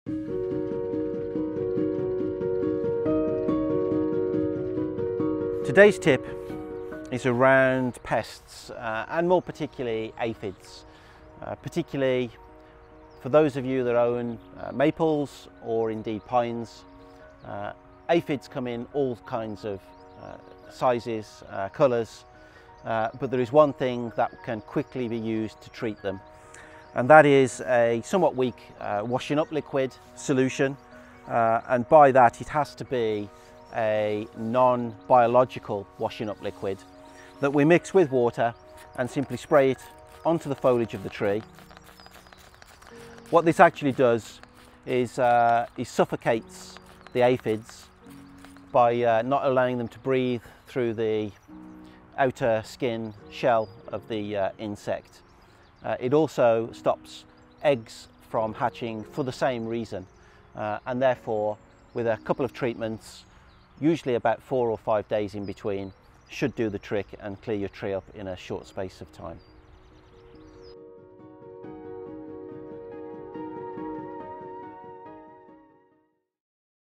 KNOWvember Bonsai Series #08 - Dealing with Aphids
KNOWvember Bonsai Series #08

KNOWvember is a series of bonsai related hints and tips running throughout November.

#08 We discuss treating your bonsai for aphid infestations